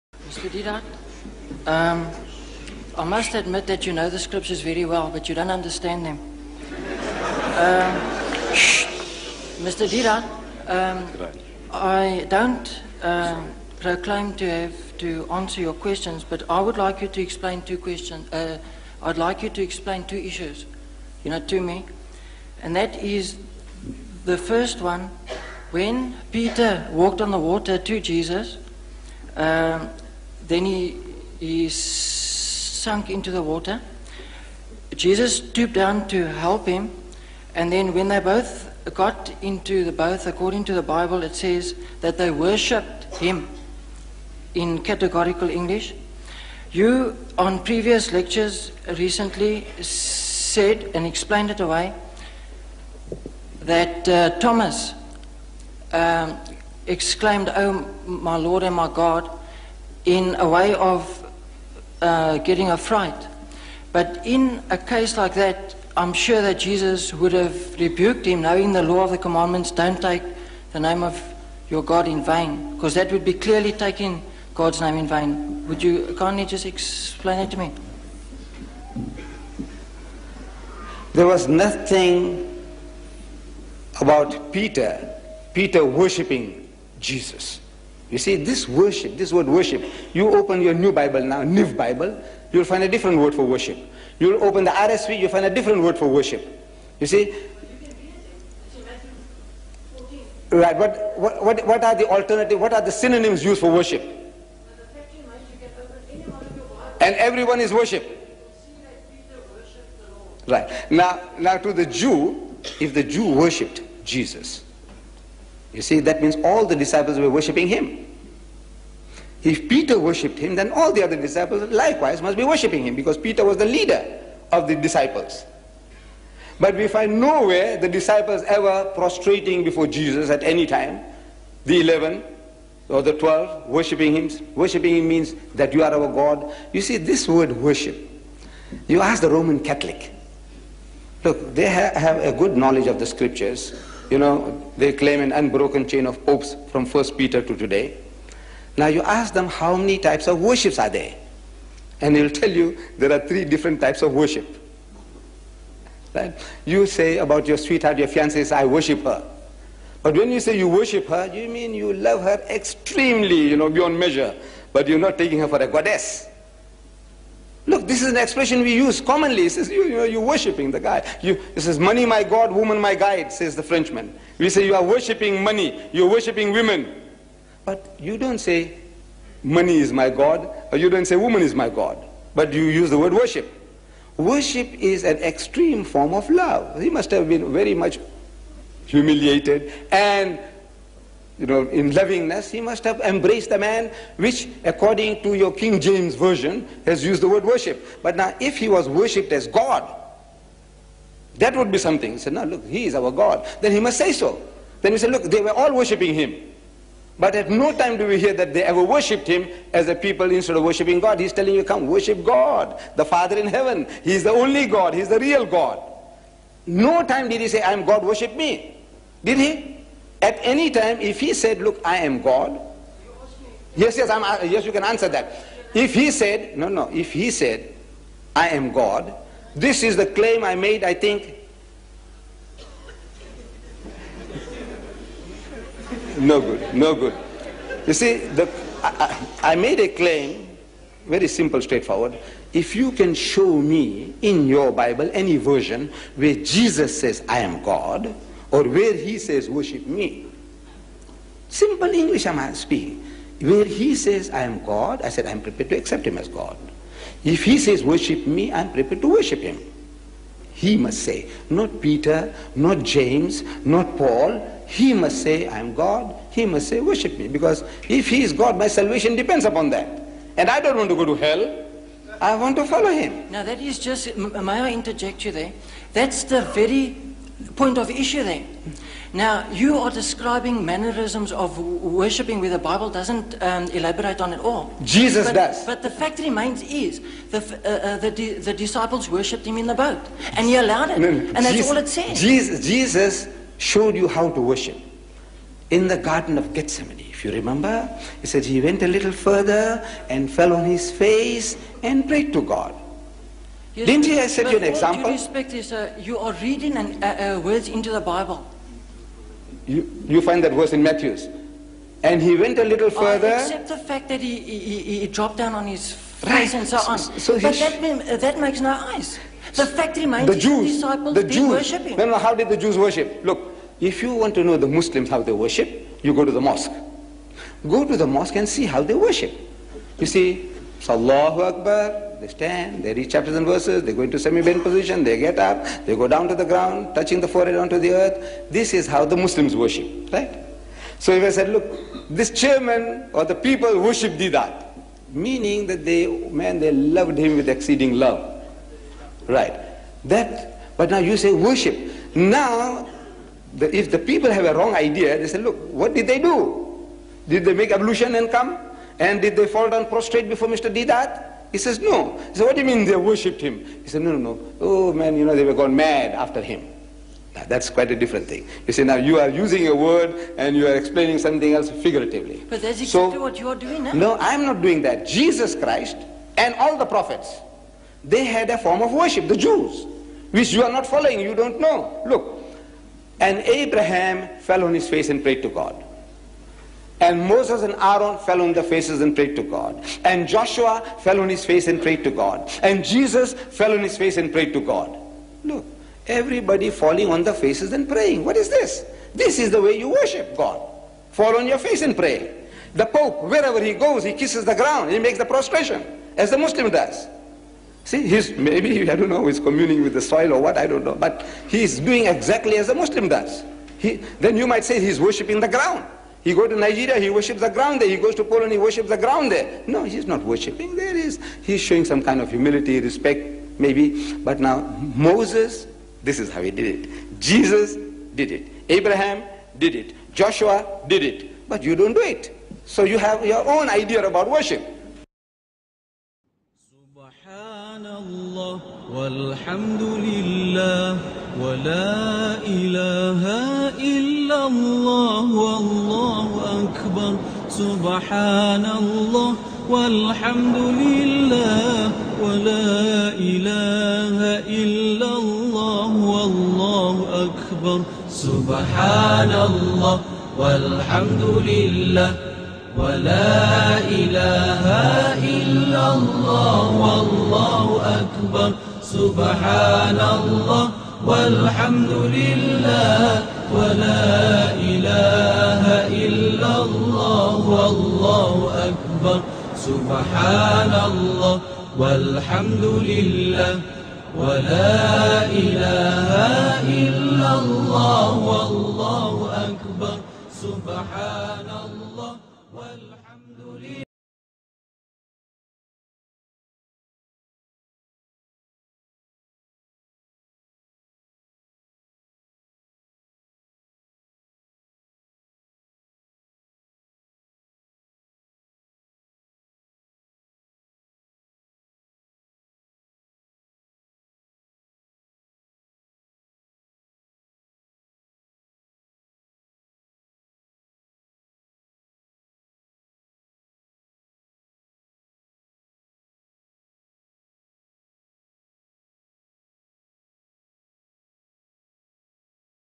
Ahmed Deedat Answer - Did the disciples 'worship' Jesus as God?
In a lecture in South Africa.

Ahmed Deedat explains that the word 'worship' in the bible is more often then not not used in the sense of worshiping God.

The Christians have mistaken this word worship to be the same. Whereas worship means also to revere, adore etc etc.

The end of the video has some explanations.

May Allah guide us all to the truth. Ameen.

Jesus peace be upon him in fact refuted people who dared to pray to him as Almighty GOD.

"And Jesus said to him, 'Why do you call me good? No one is good but God alone. (From the NIV Bible, Mark 10:18)"

"Here, O Israel: The Lord our God is one Lord. (From the NIV Bible, Mark 12:29)"

"My Father (GOD) is greater than I. (From the NIV Bible, John 14:28)"

"If I testify about myself, my testimony is not valid. (From the NIV Bible, John 5:31)"

"By myself I can do nothing; I judge only as I hear, and my judgment is just, for I seek not to please myself but him who sent me. (From the NIV Bible, John 5:30)"

"...You have never heard his (GOD's) voice nor seen his form...(From the NIV Bible, John 5:37)"

"Father (GOD), into thy hands I commend my spirit. (From the NIV Bible, Luke 23:46)"

"No one knows about that day or hour, not even the angels in heaven, nor the Son, but only the Father. (From the NIV Bible, Matthew 24:36)" Not even Jesus knows when the Hour will come! How in the World can he be the Creator of the Universe?!